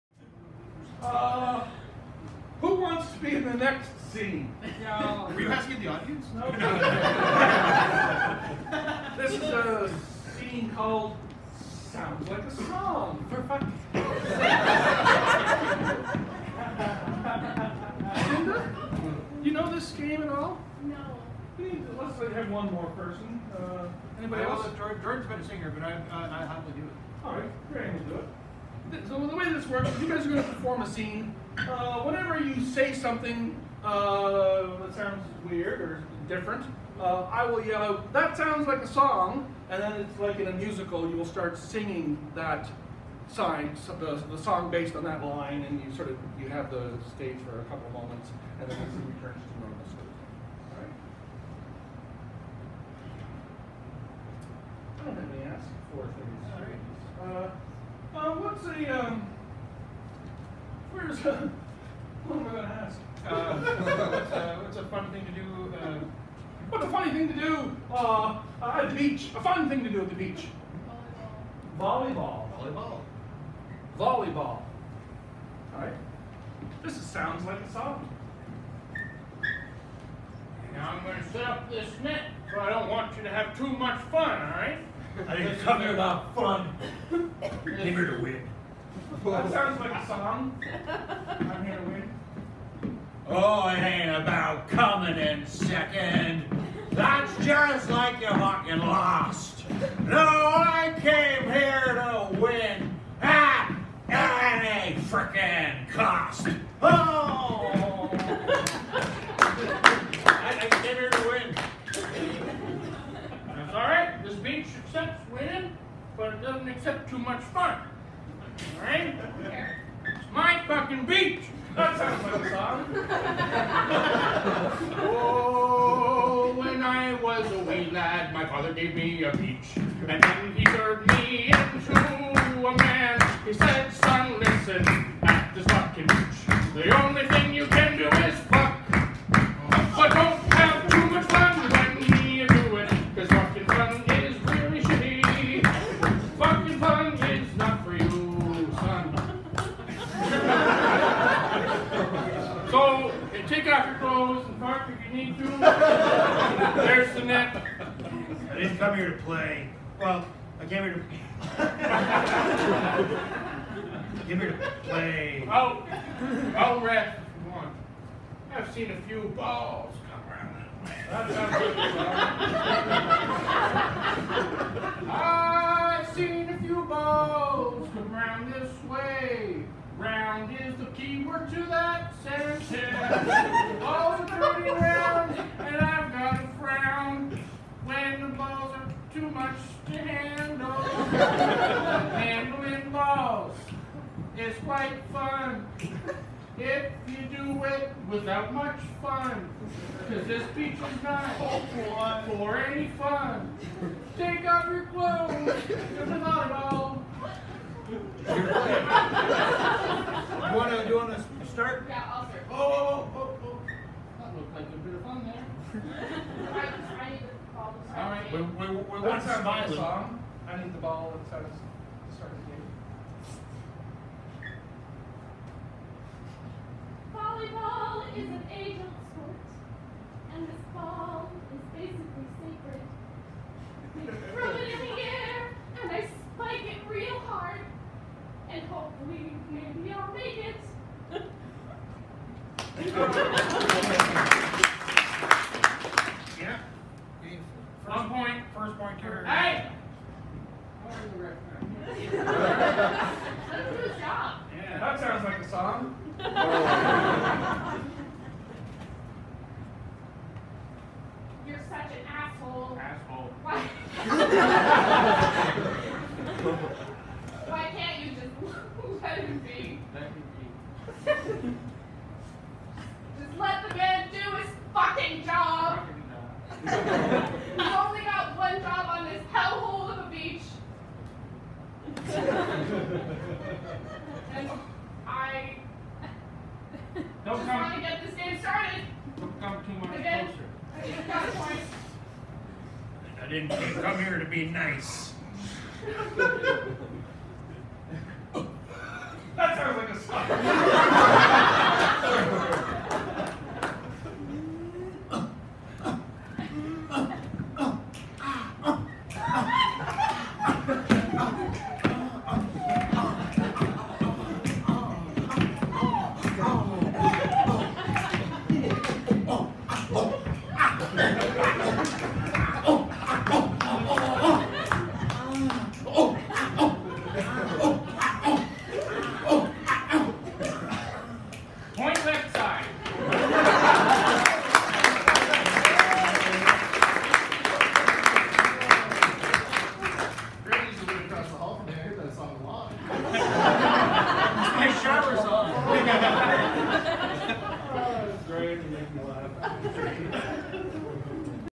First 2 Hours Sounds Like A Song 9 41pm
The Guild presents Popalopalots Improv Comedy 26 Hour Marathon Fundraiser for the QEH Foundation with title sponsor Coldwell Banker Parker Realty - March 30 8pm - March 31 10pm